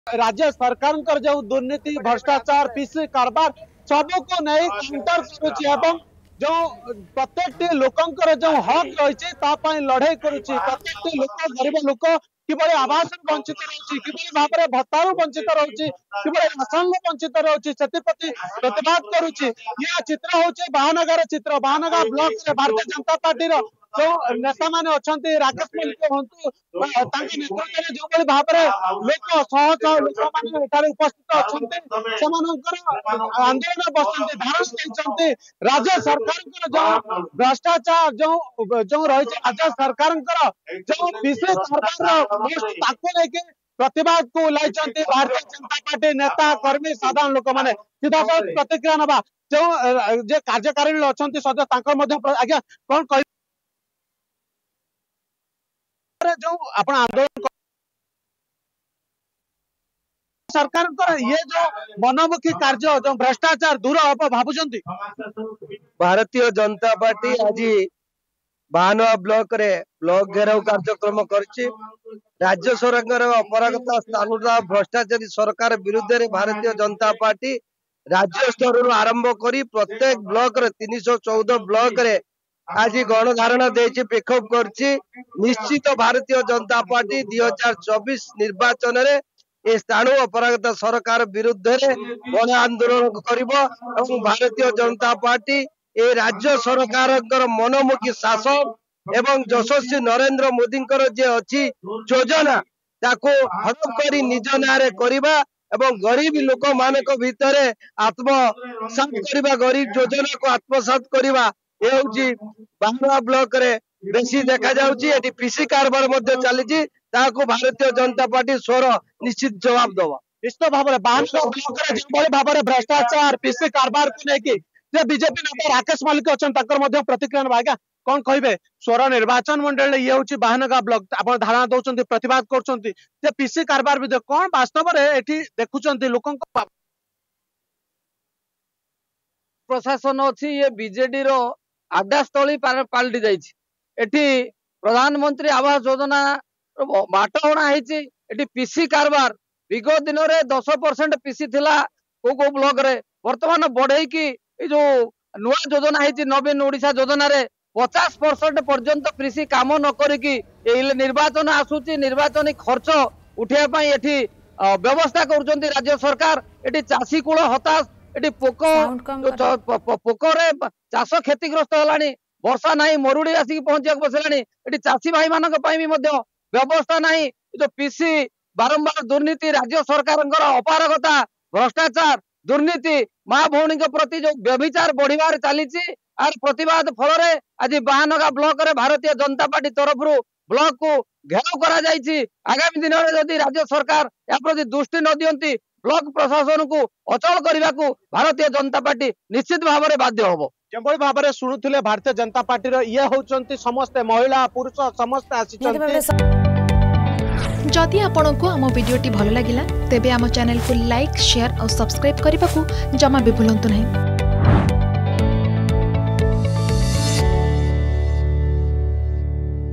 ବାଲେଶ୍ବର ବାହାନଗାରେ ବିଜେପିର ଜନଆକ୍ରୋଶ ଆନ୍ଦୋଳନ || Balasore BJP Protest
ବାଲେଶ୍ବର ବାହାନଗାରେ ବିଜେପିର ଜନଆକ୍ରୋଶ ଆନ୍ଦୋଳନ । 24 ବର୍ଷର ବିଜେଡି ସରକାରର ବିଫଳତା ଓ ଅପାରଗତାର ପୋଲ ଖୋଲିବାକୁ ବିଜେପିର ବଡ଼ ପ୍ଲାନ୍ ।